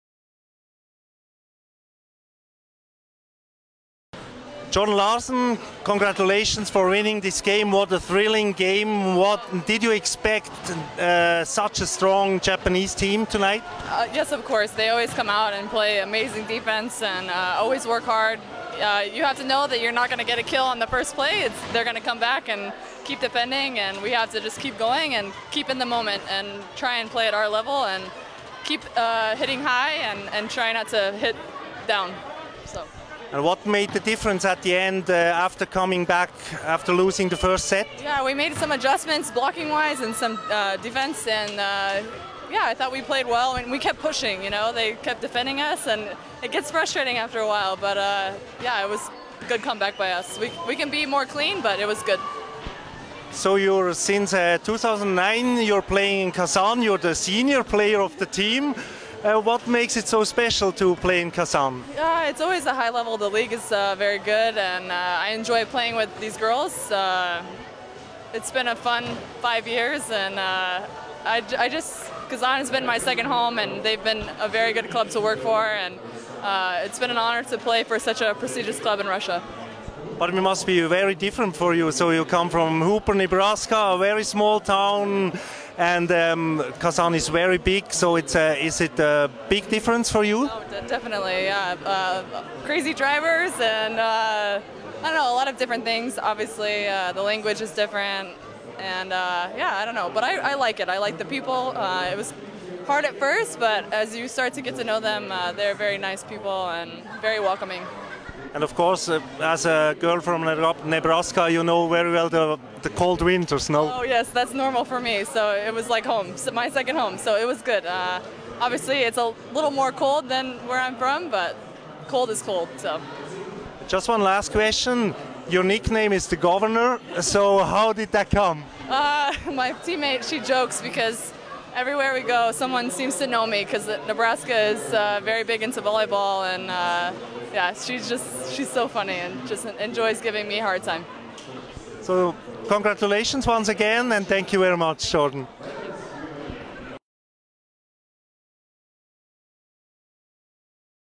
Between the USA and Russia
Jordan Larson-Burbach tells how it is to live the "American way of life" in Russia.